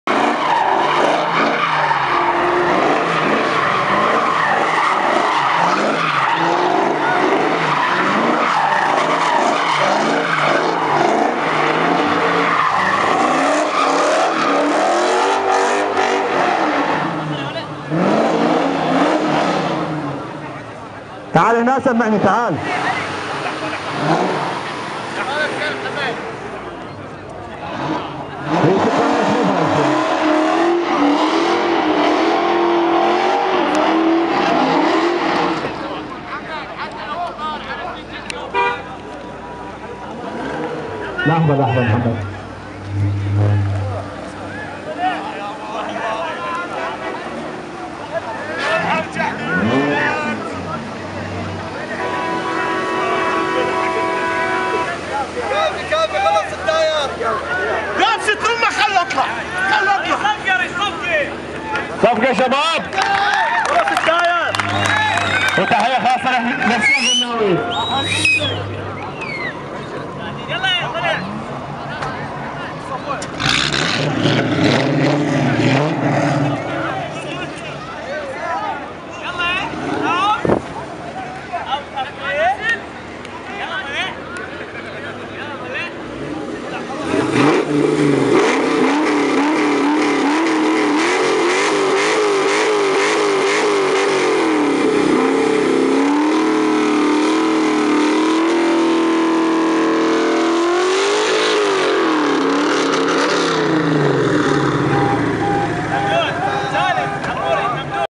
استعراض مصطفى الضلع في نادي الفروسية
استعراض مصطفى الضلع في نادي الفروسية في 1/8/2014
في بغداد بتنظيم فريق Tigers Team